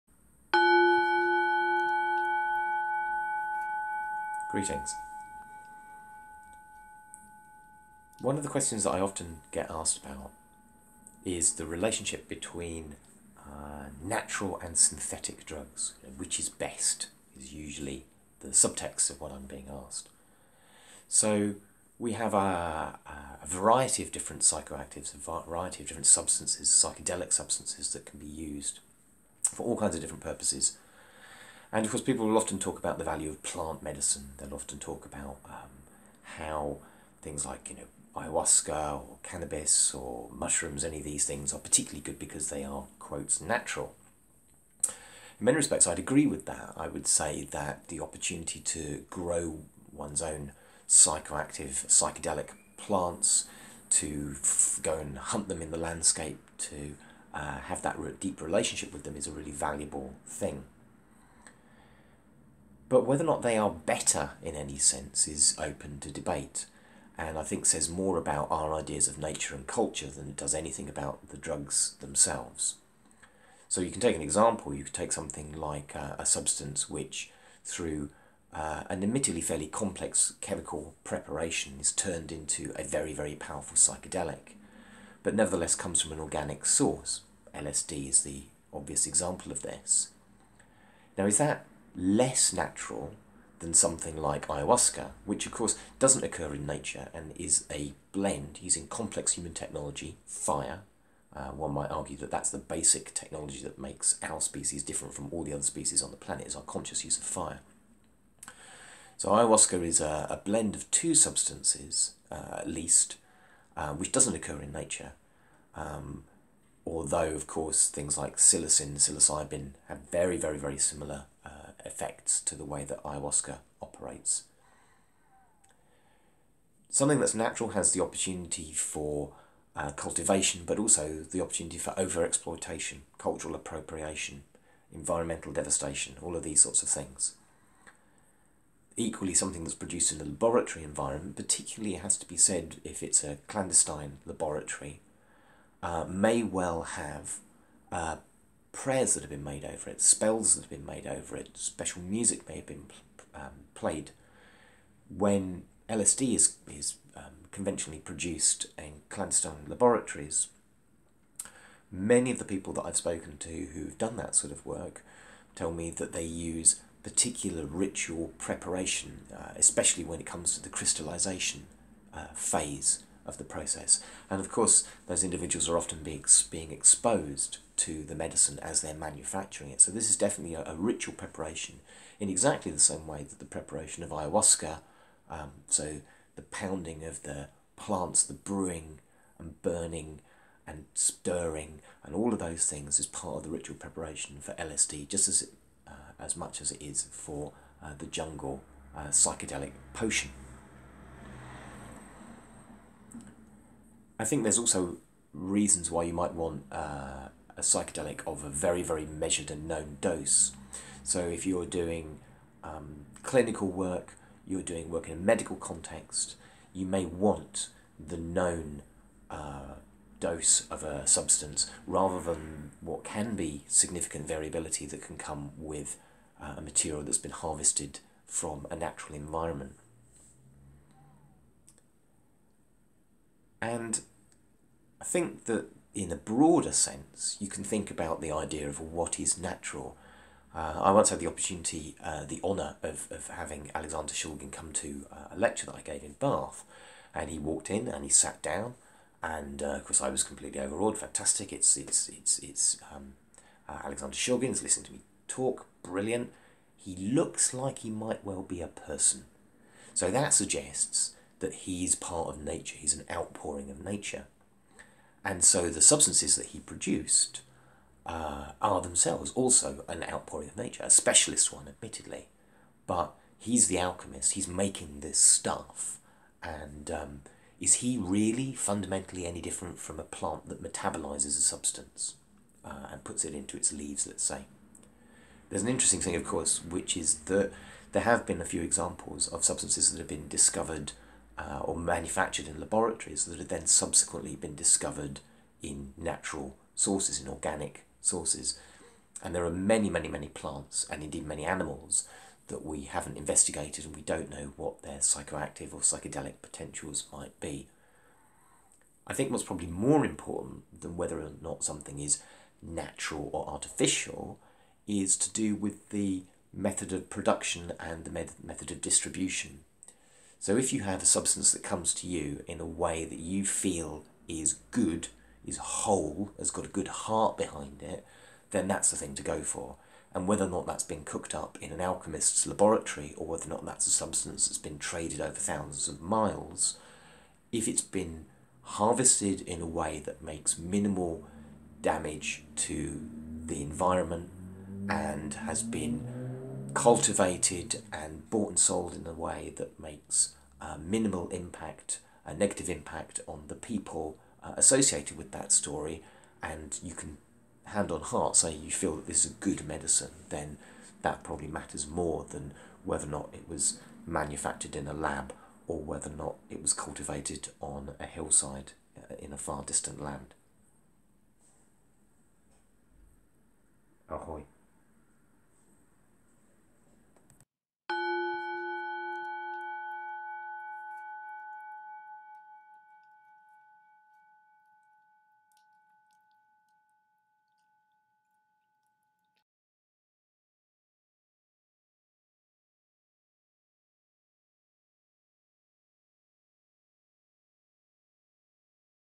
Natural or Artificial Psychedelics
A few thoughts on the relationship between ideas of nature and culture and our choice of psychedelic drugs.